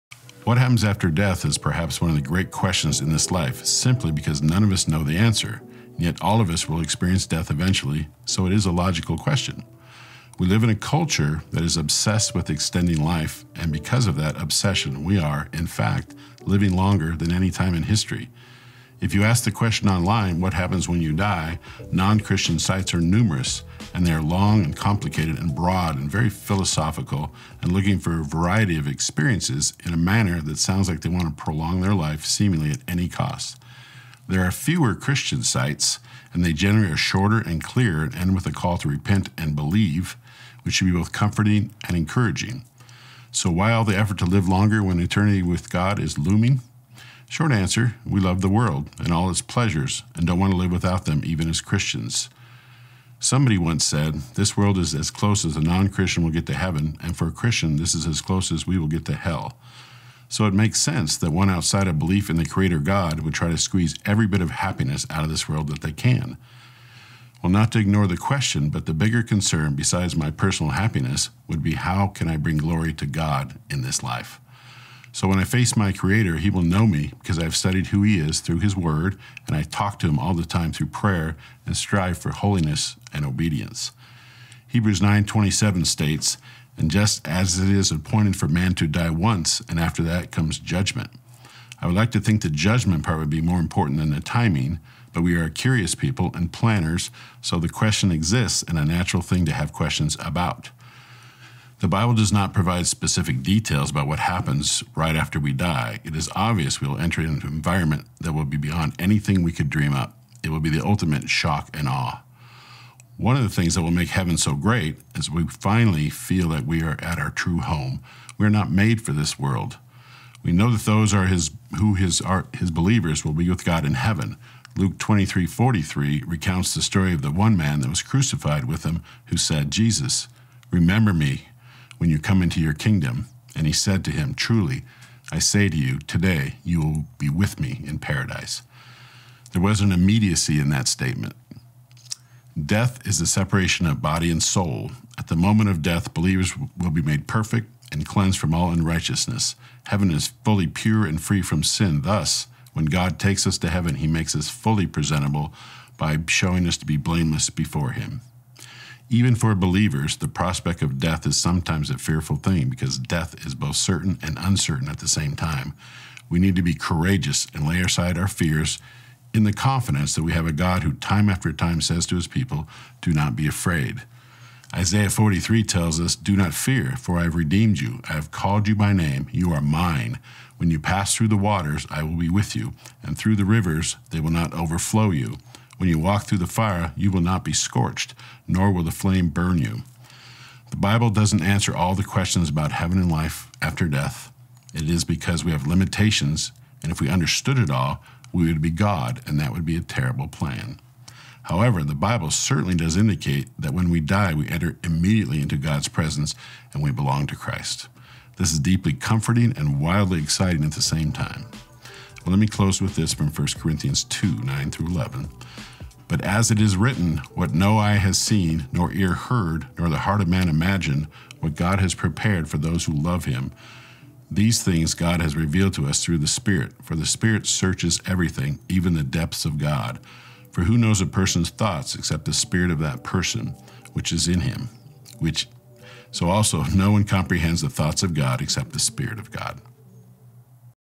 What Happens After Death? | Ask Redeemer | Tim Anderson
Pastor Tim Anderson answers the question, "What happens after death?"

For more information about Redeemer Bible Church in Gilbert, Arizona or to help support this ministry, please visit us